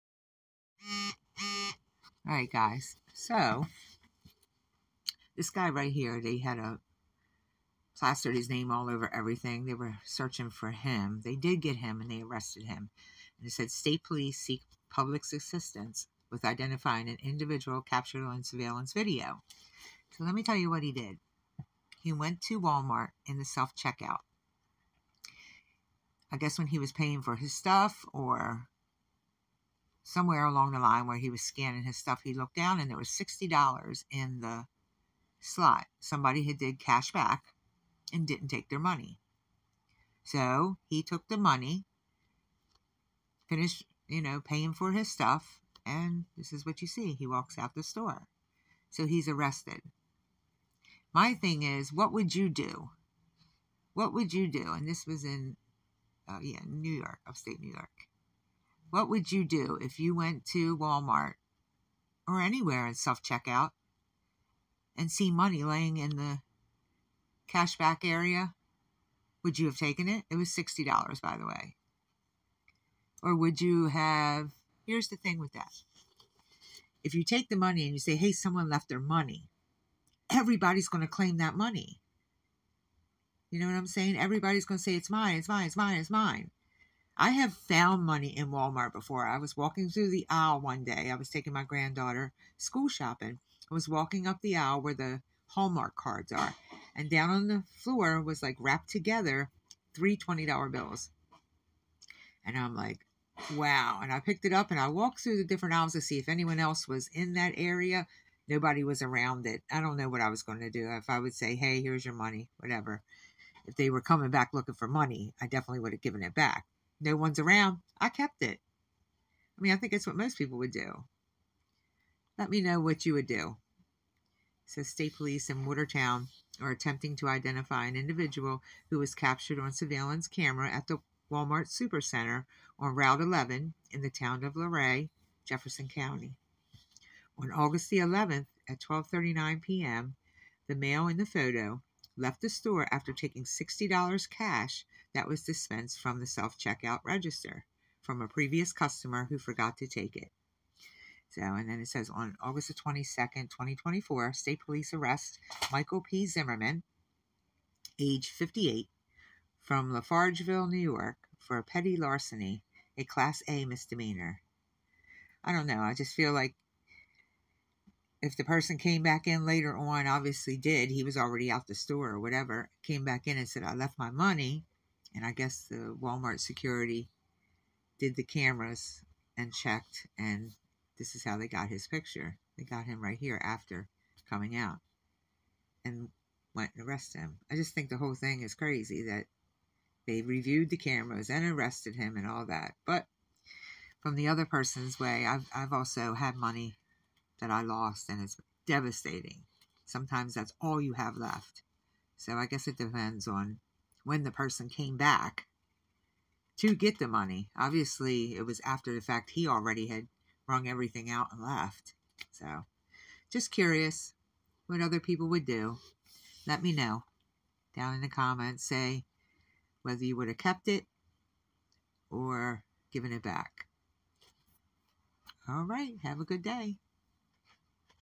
WHAT WOULD YOU DO if you found MONEY?
Michael P. Zimmerman age 58 was arrested for check out theft. in Le Ray, NY Super Walmart. I've added a link to the story below ⏬